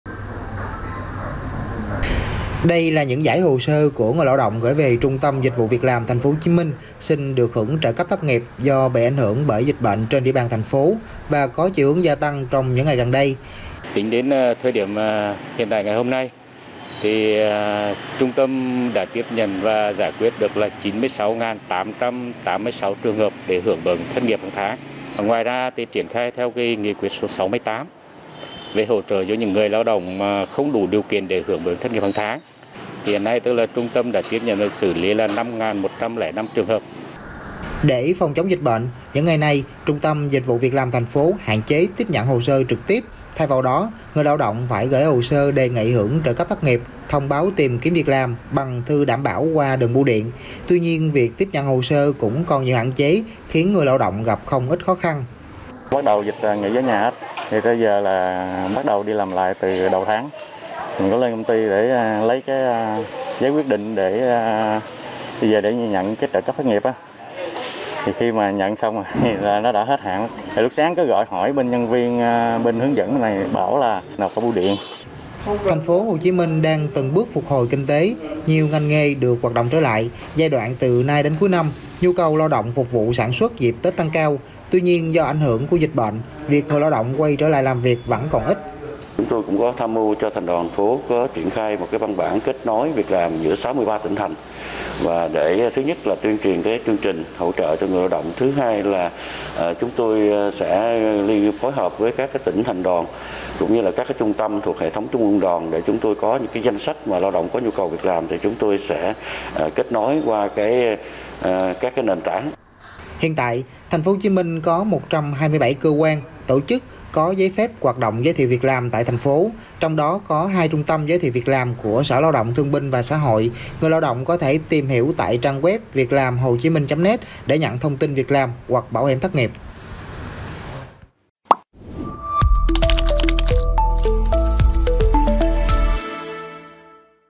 Hồ sơ bảo hiểm thất nghiệp tăng cao tại TP. Hồ Chí Minh | VTV24
Tính từ đầu năm đến nay tại Trung tâm Dịch vụ việc làm TP.HCM đã tiếp nhận gần 97.000 hồ sơ người lao động đề nghị hưởng trợ cấp thất nghiệp. Ngoài ra, việc hỗ trợ người lao động tìm việc cũng tăng cao trong thời gian gần đây.